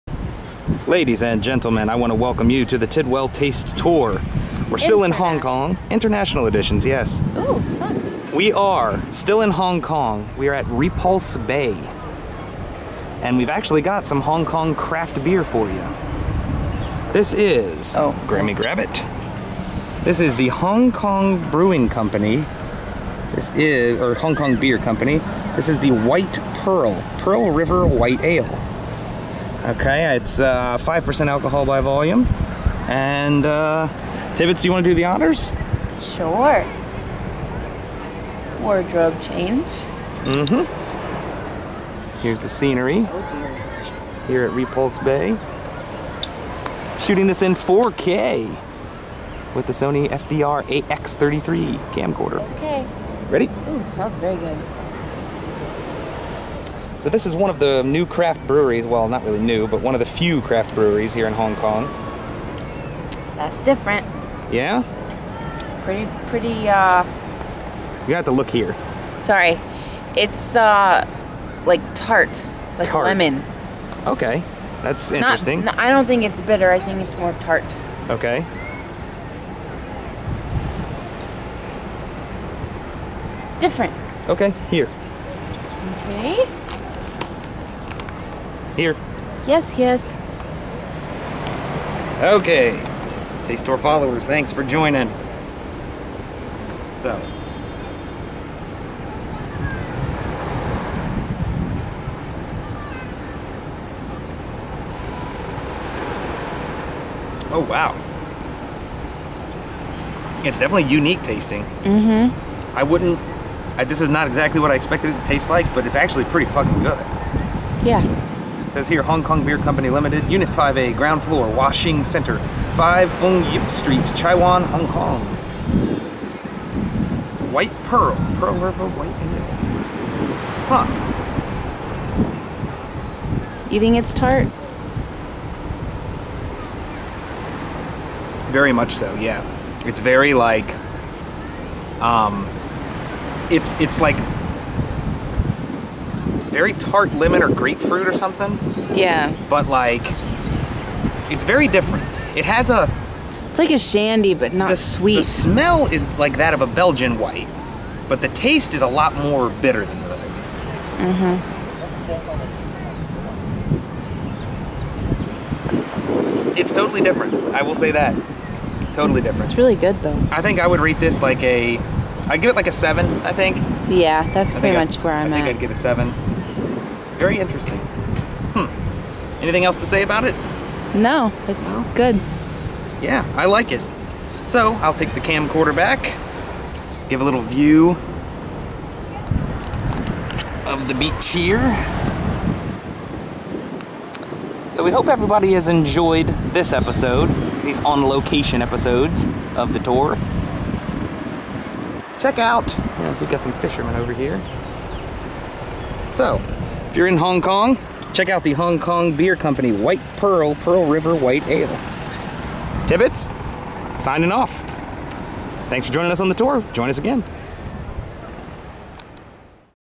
Hong Kong Beer Co. White Pearl Review - Pearl River White Ale - Repulse Bay Beach, Hong Kong - 4K
Welcome back to the Taste Tour everyone! This time around on the international editions - Repulse Bay in Hong Kong is the setting, overlooking the warm and blue South China Sea! This time around we're trying out a microbrew - Hong Kong Beer Co., a local original, called White Pearl (Pearl River White Ale). The website description reads:

WHITE PEARL
TASTING NOTES:
Our interpretation of a Belgian white ale has a decidedly "Pearl River Delta" character. The generous amount of wheat malt, a bevy of local ingredients (including Mandarin orange peel, rose buds and honey) plus our special yeast give this pale straw-colored, unfiltered beer a refreshingly complex tart and citrusy flavor with a hint of honey and infused aromatics.

SUGGESTED FOOD PAIRINGS:
Classic with steamed mussels but also great with light seafood dishes, salads, sushi, grilled chicken, herbed cheeses and many citrusy desserts.

Cheers!